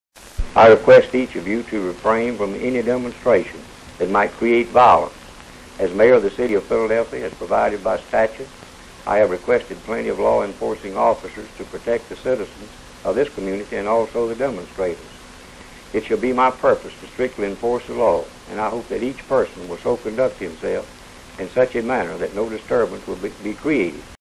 Mayor of Philadelphia Clayton Lewis, 1966. Film 91190
Clayton Lewis.  Civil Rights Movement in the 1960s.
Mayor of Philadelphia, Clayton Lewis, making statement in radio station for broadcast.  He appeals for calm and states that law officers are there to protect all involved.  1966